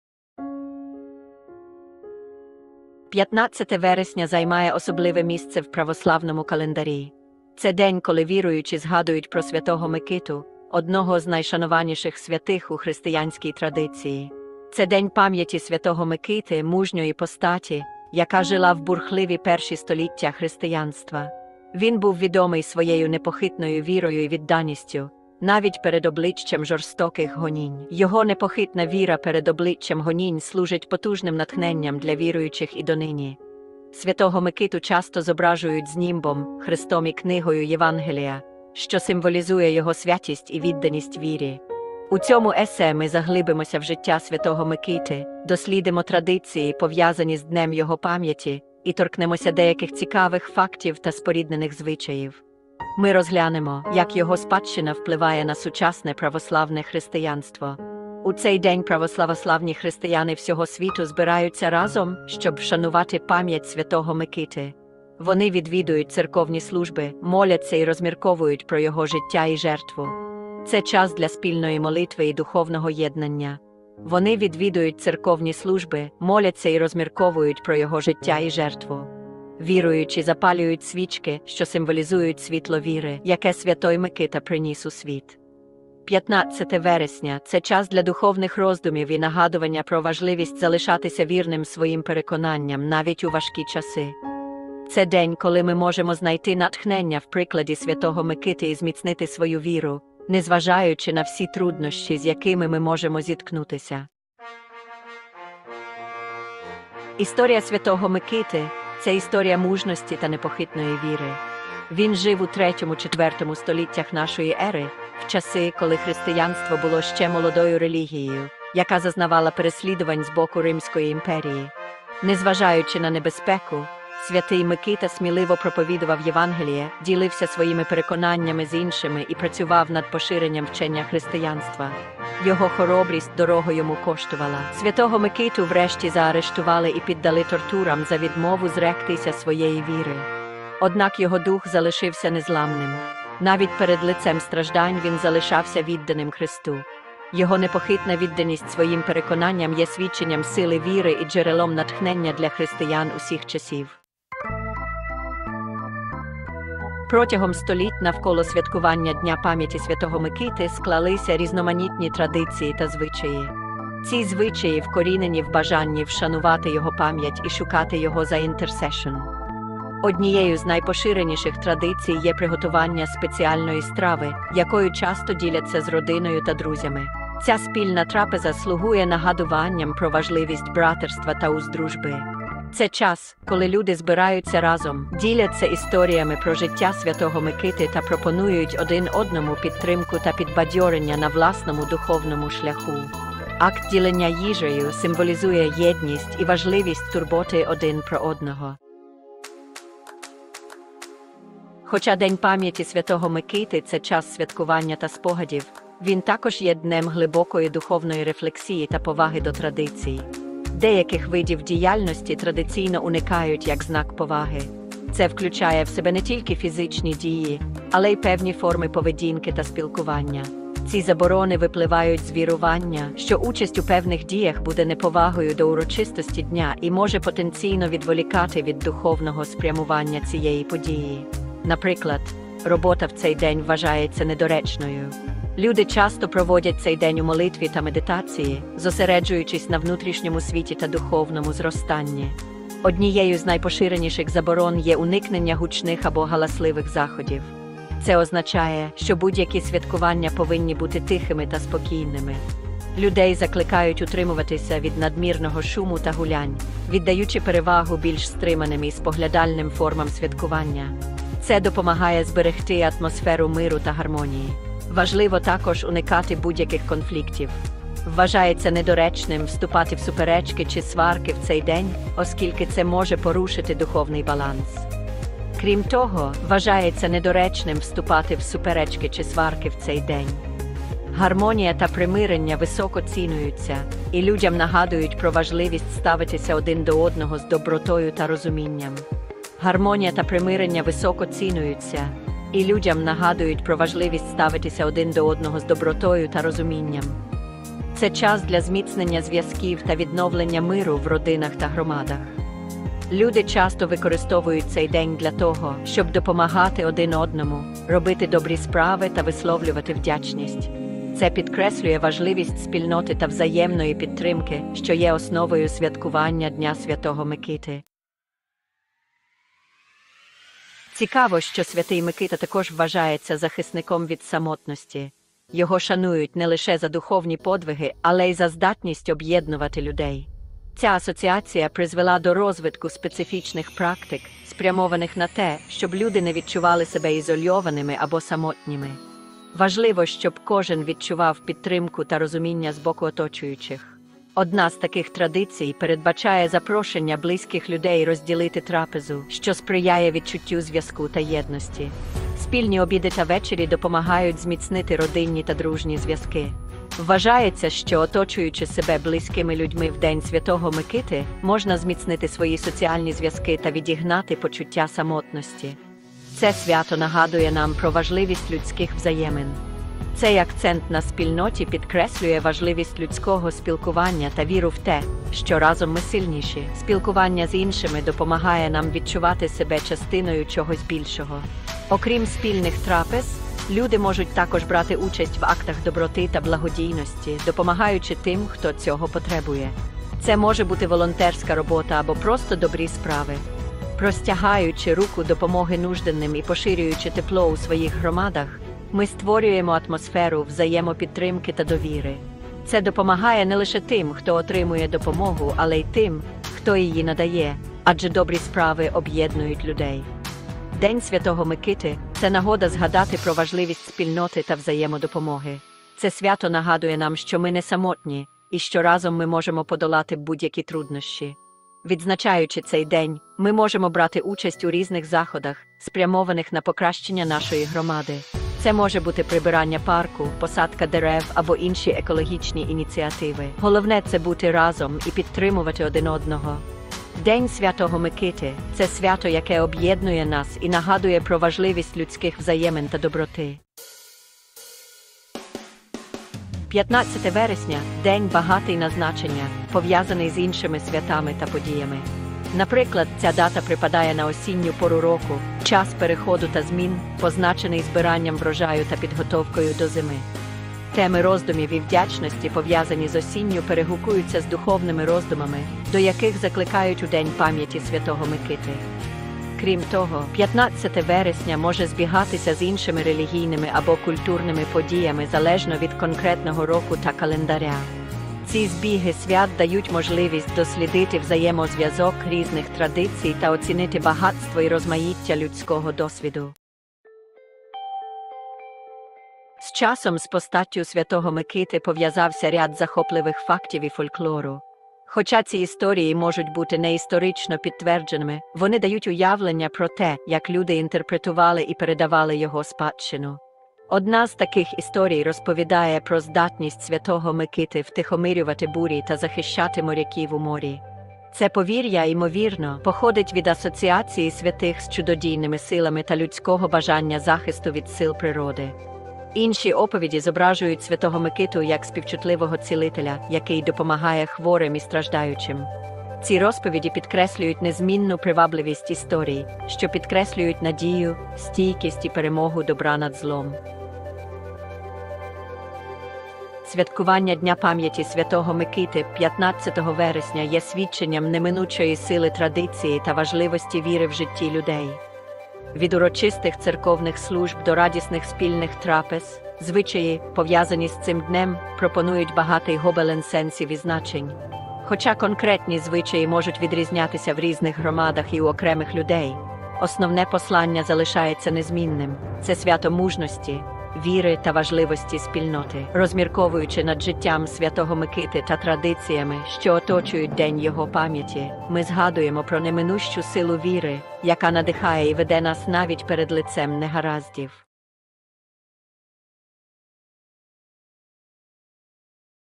Церковне свято 15 вересня 2024: молитви, прикмети та
OUTLINE:
00:00:00 Introducing Saint Nikita's Day
00:01:44 The Life and Legacy of Saint Nikita
00:02:33 Traditions and Customs
00:03:18 Prohibitions on Saint Nikita's Day
00:05:17 Activities for September 15th
00:07:23 Related Observances
00:08:06 Facts and Folklore
00:08:53 The Enduring Significance of Tradition

🌟 Друзі, 15 вересня 2024 року ми святкуємо важливе церковне свято в Україні! 🎉 У цьому відео ми розповімо вам про святий день пам'яті великомученика Микити, його життєвий шлях та значення для віруючих. 🙌

🔍 Що потрібно знати про це свято?
- Кому моляться віряни 🕊️
- Народні прикмети для 15 вересня 🍂
- Заходи, які не можна робити в цей день 🚫

💡 Дізнайтеся, як підтримати своїх близьких та друзів, щоб не залишитися самотніми в цей важливий день. Не забудьте приготувати страву з м'ясом гуски, щоб залучити позитивну енергію у ваше життя! 🍽️✨

📅 Приєднуйтесь до нас у вивченні традицій та звичаїв, що оточують це свято, та діліться своїми думками у коментарях! 💬